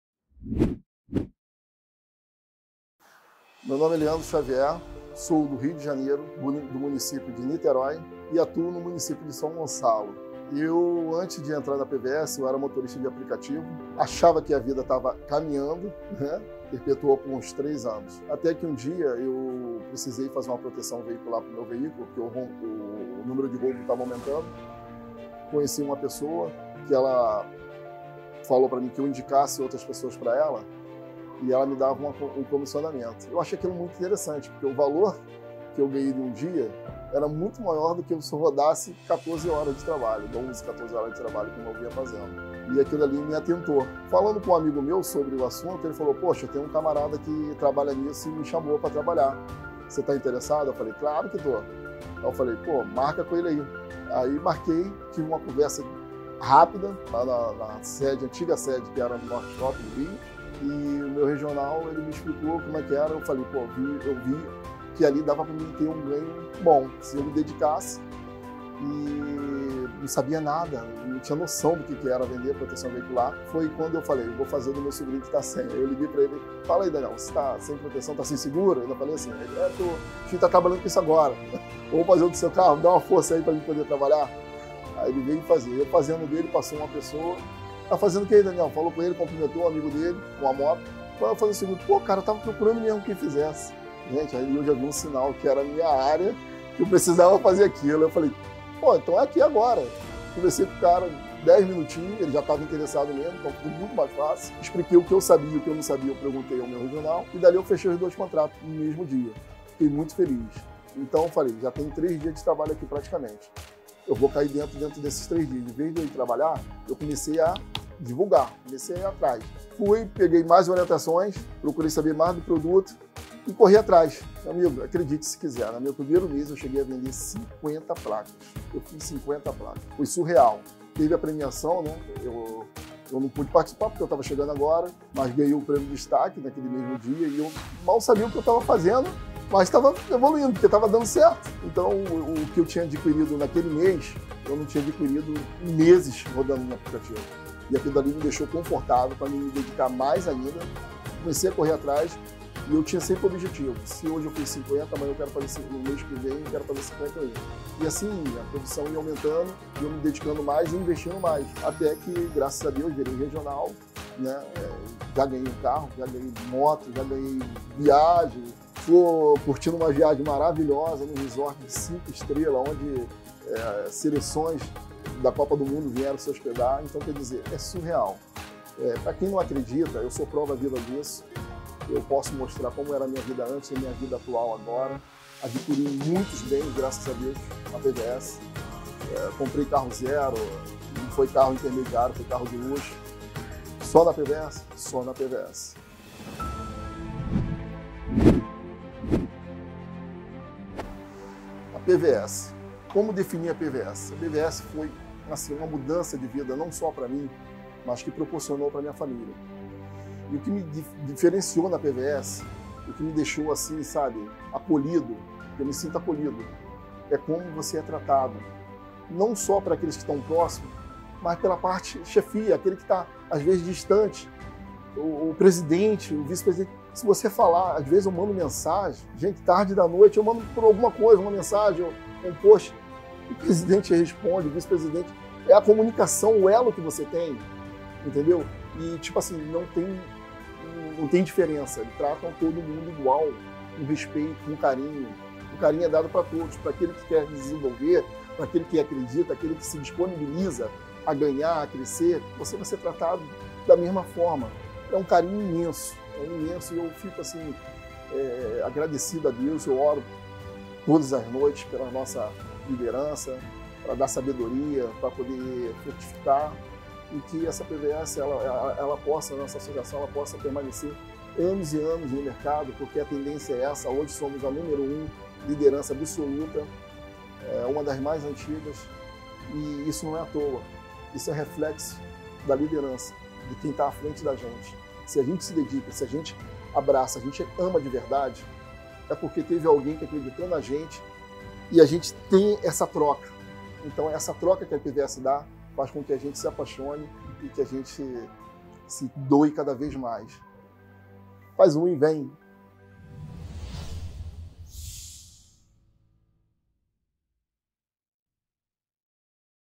Depoimento - Leandro Xavier
Saiba mais sobre o caminho de sucesso do Regional Leandro Xavier dentro da APVS Brasil.